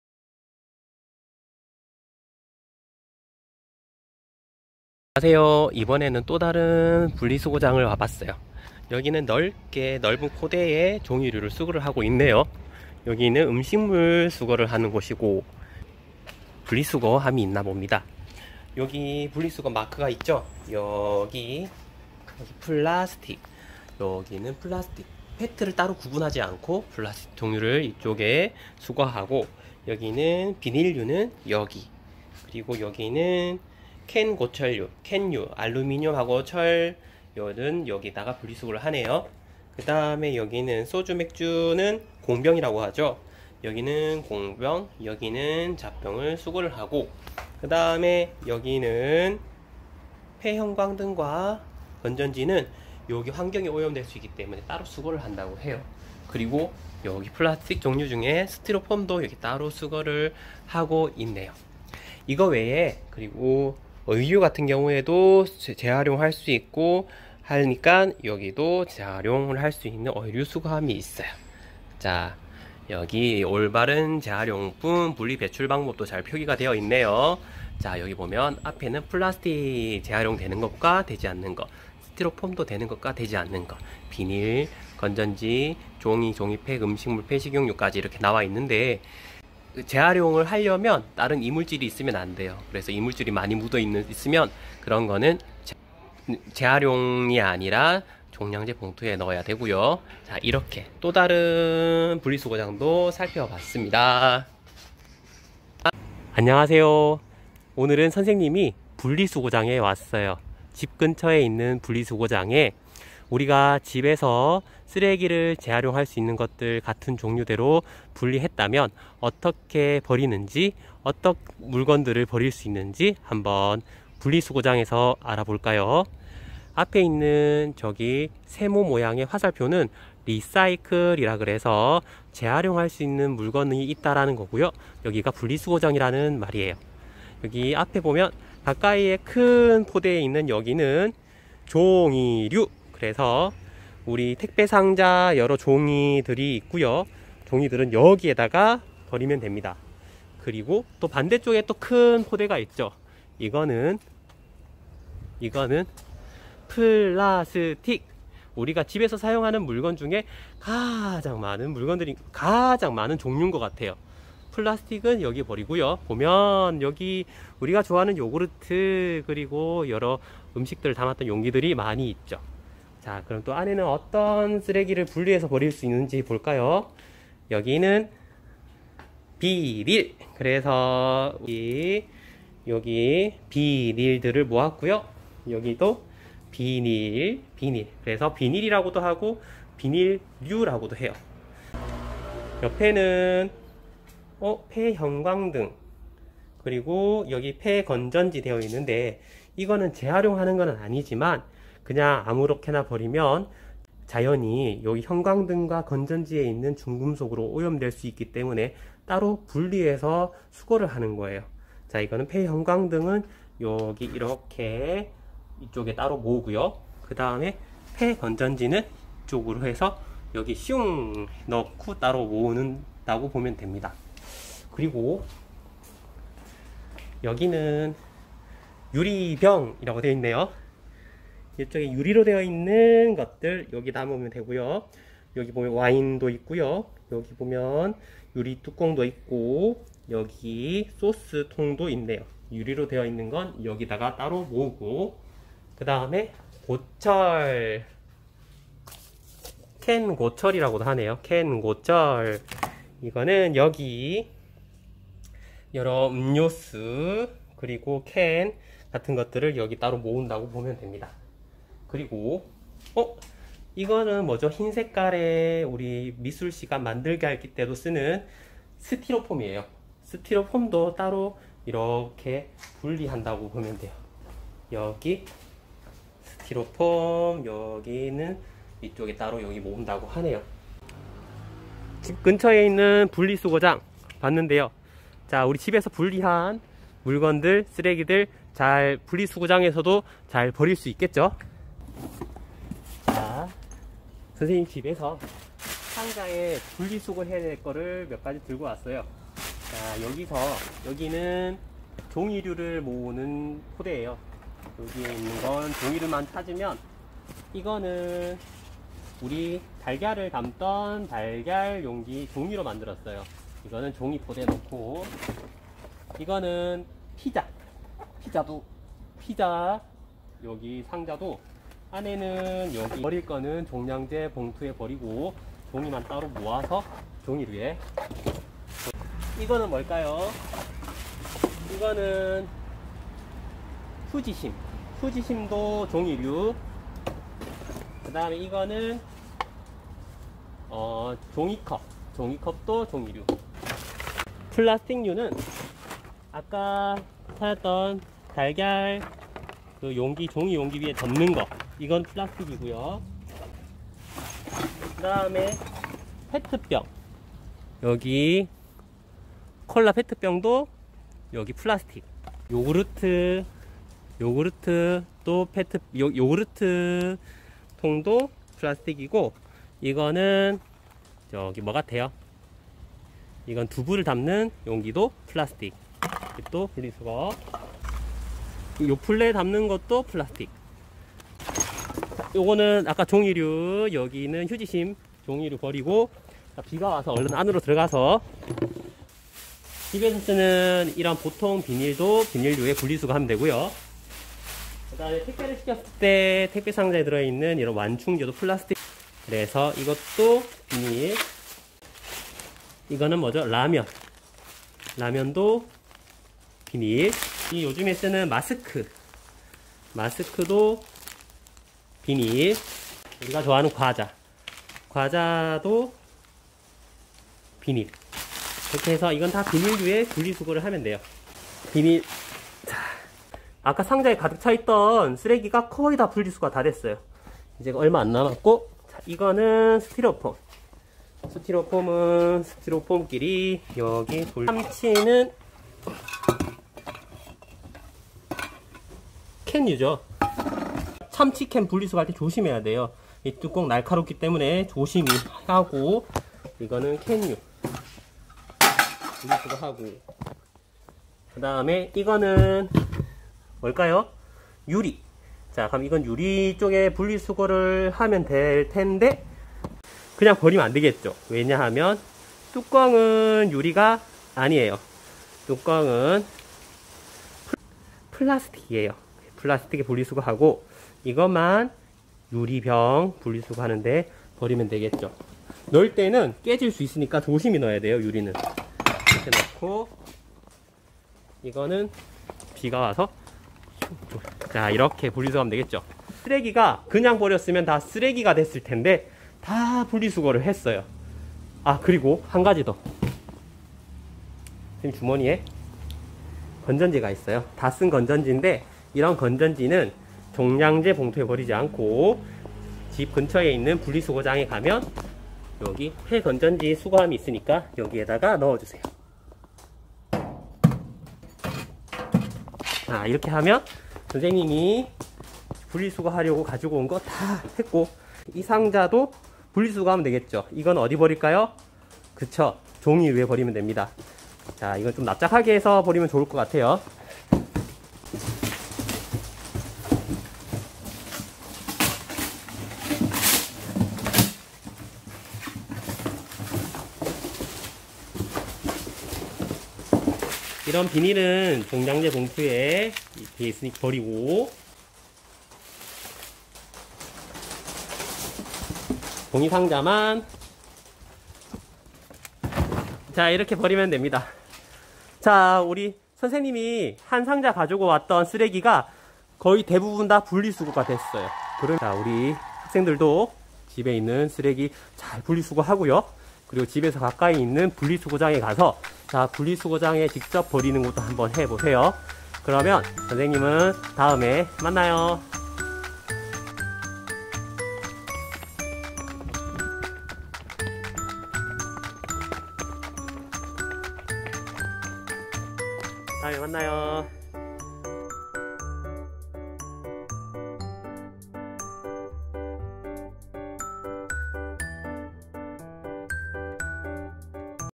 용용샘과 환경교육 | #5 우리 동네 분리수거장 | 재활용 쓰레기 분리배출
용용샘과 환경교육
#5 우리 동네 분리수거장

동네에서 볼 수 있는 분리수거장에서
무엇을 수거하고 있는지 살펴봅시다.

집에서 버리는 쓰레기 직접 버려보면서
우리 동네에서는 분리수거가 어떻게 되고 있는지 살펴보세요!